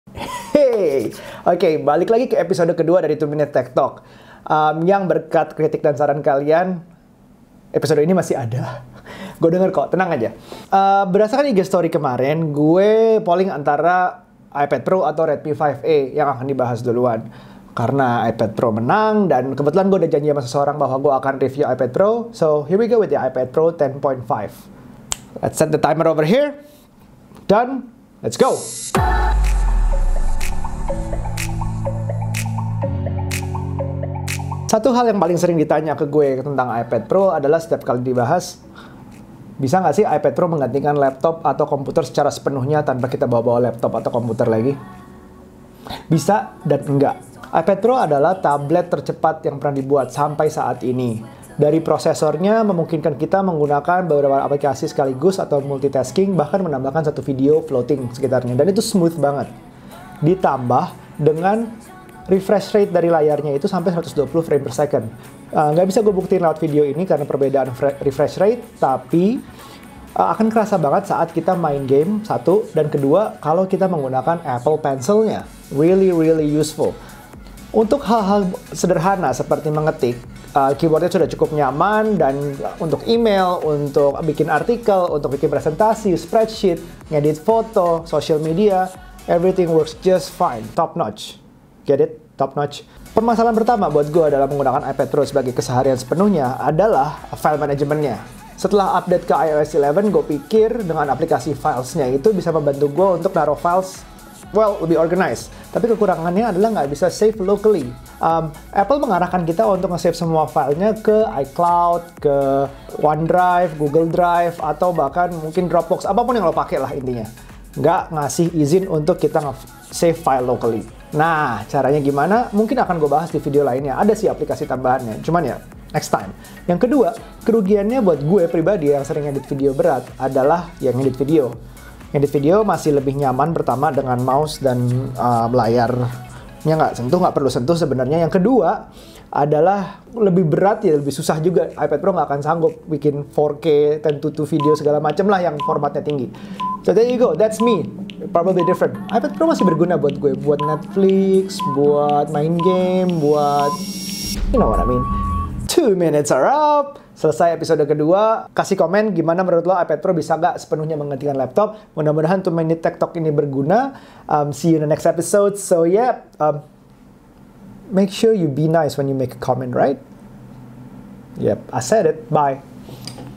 Bisakah IPAD PRO Menggantikan Laptop?
Bisakah iPad Pro Menggantikan Laptop? Pertanyaan paling sering ditanyakan ke saya, dan ini jawabannya.

Untuk menghemat waktu, ini spesifikasinya.

Retina display
10.5-inch (diagonal) LED-backlit Multi-Touch display
2224-by-1668 resolution at 264 pixels per inch (ppi)
A10X Fusion chip with 64‑bit architecture
Embedded M10 coprocessor

12-megapixel camera
ƒ/1.8 aperture
Digital zoom up to 5x
Optical image stabilization

4K video recording at 30 fps
1080p HD video recording at 30 fps or 60 fps
720p HD video recording at 30 fps
Optical image stabilization for video

Built‐in 30.4‐watt‐hour rechargeable

Logo by: @holisyit

on ask.fm/sheggario
on @sheggario